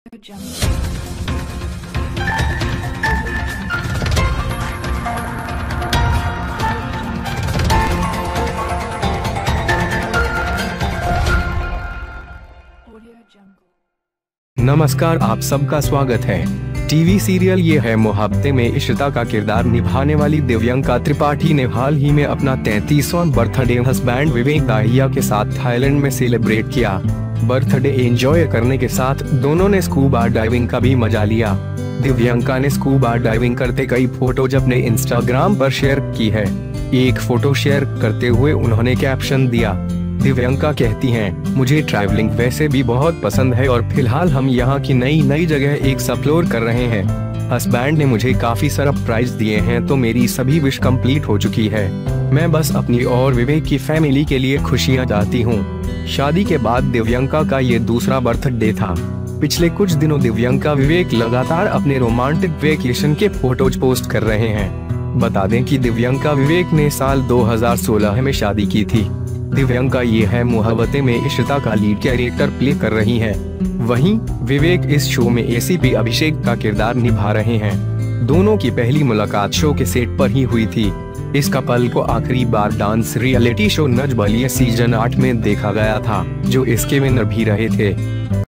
हसबैंड के साथ स्कूबा डाइविंग करती दिखीं टीवी की इशिता | Divyanka Tripathi Vivek Dahiya In Thailand
Divyanka Tripathi, who played Ishita in the TV serial 'Ye Hai Mohabbaten', recently celebrated her 33rd birthday with Hasaband Vivek Dahiya in Thailand. Both enjoy scuba diving as they enjoy the birthday. Divyanka shared several photos scuba diving on her Instagram.
Divyaanka says, "I love traveling well anyway and at the moment we are exploring new places here." Husband has given me a lot of surprise, so all my wishes have been completed, I am just concerned about my own discretion. I want to be happy. " This was the second birthday of Divyanka after marriage. Divyanka-Vivek has been constantly posting photos of her romantic vacation for the last few days.